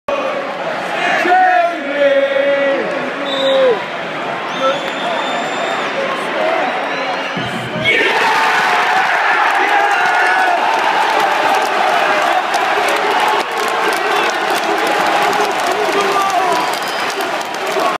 Sheffield Wednesday Vs Derby County
Jamie ward puts Derby 2-0 up with a 2nd half penalty at hillsborough 9/2/13